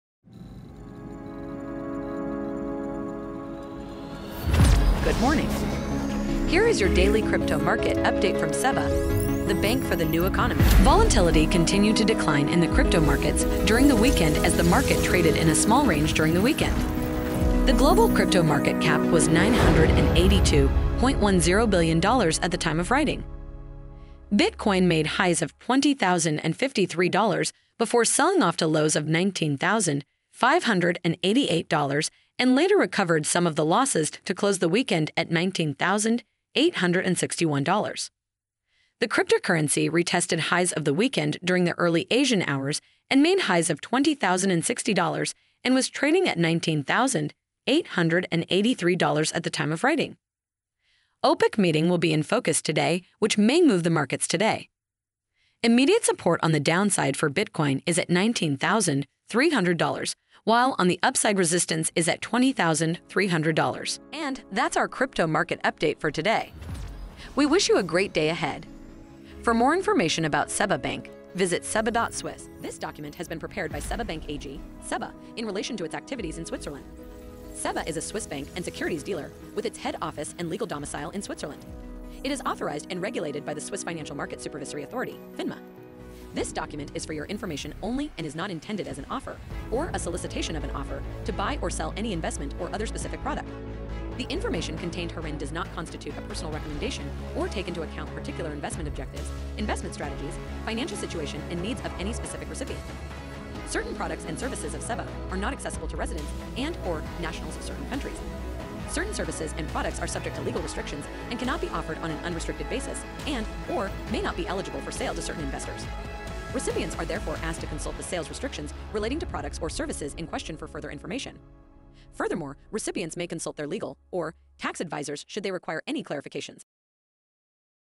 Daily Crypto Market Update - 05.09.2022
SEBA Bank | Daily Crypto Market Report Daily Rundown – BTC, ETH and DOT

 Volatility continued to decline in the crypto markets during the weekend as the market traded in a small range during the weekend. The global crypto market cap was $982.10B at the time of writing.

 Bitcoin made highs of $20,053 before selling off to lows of $19,588 and later recovered some of the losses to close the weekend at $19,861. The cryptocurrency retested highs of the weekend during the early Asian hours and made highs of $20,060 and was trading at $19,883 at the time of writing. OPEC meeting will be in focus today which may move the markets today.

 Immediate support on the downside for Bitcoin is at $19,300 while, on the upside resistance is at $20,300.